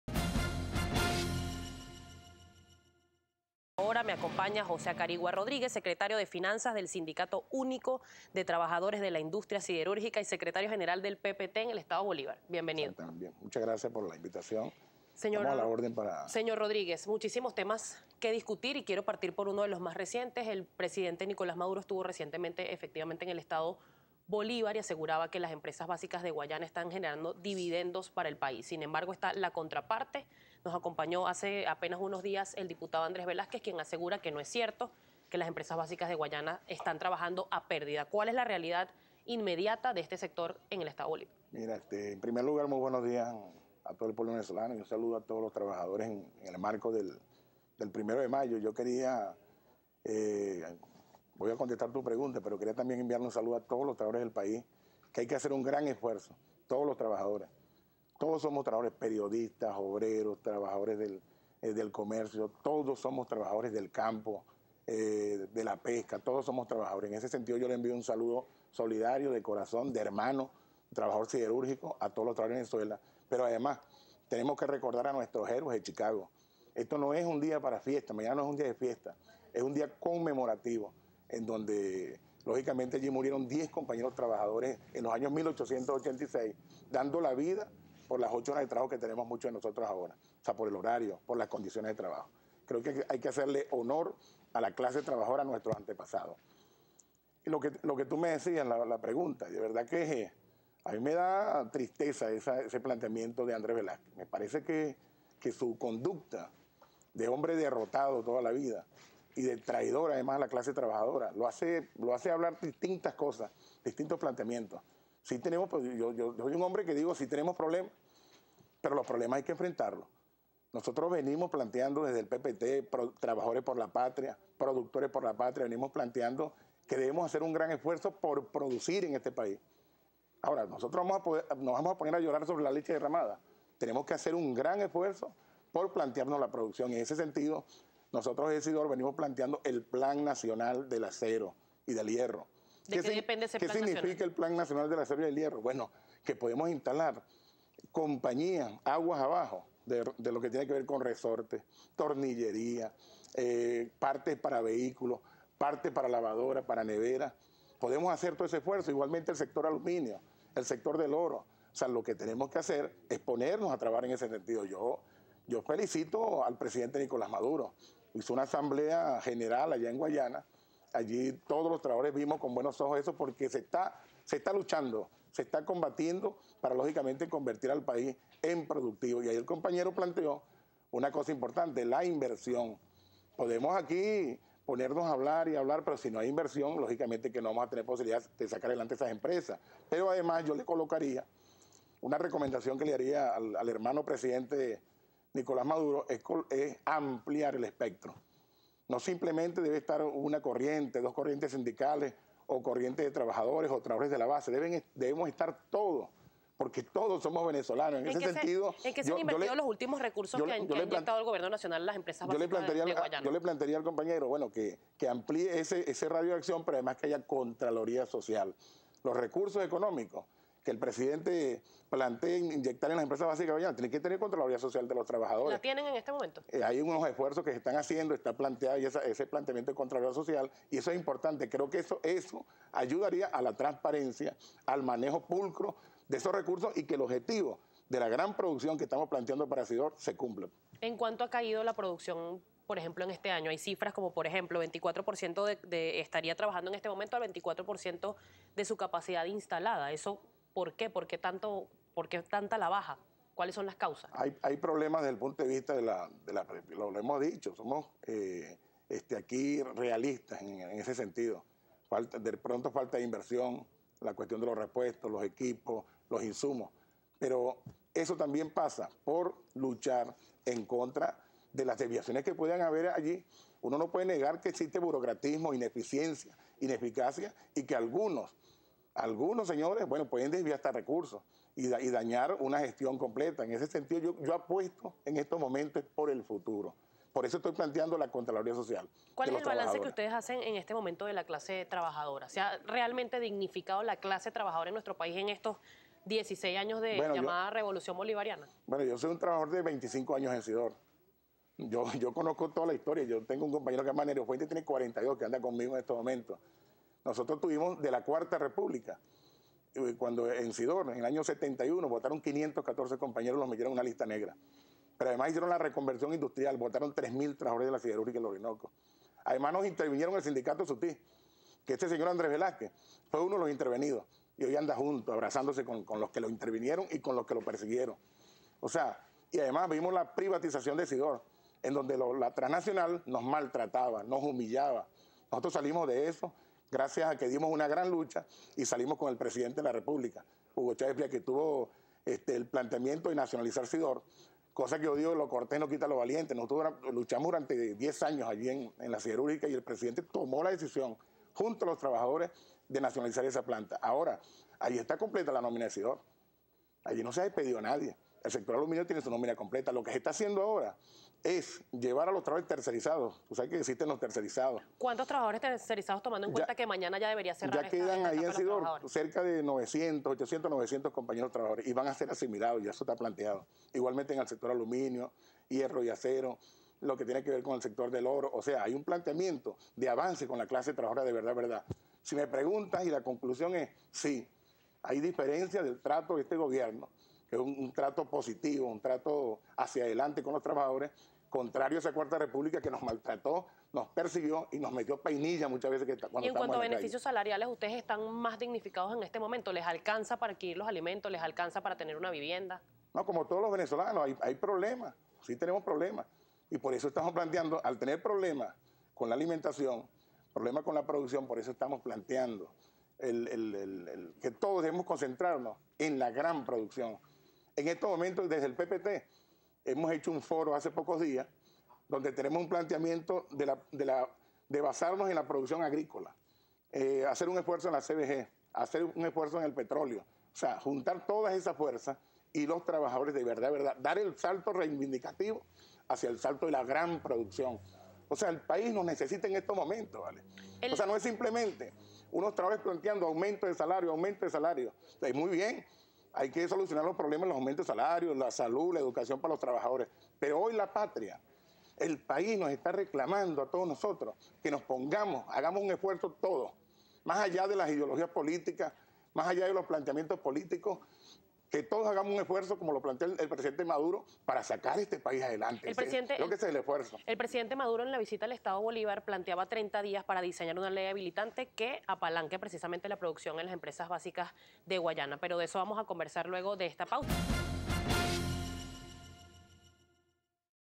Sutiss: El país nos está pidiendo un esfuerzo de producción
El secretario de finanzas de Sutiss, José Acarigua Rodríguez, manifestó que en el país hay problemas, pero lo que se impone es que todos hagan un esfuerzo y que los trabajadores deben concentrarse en “la gran producción”.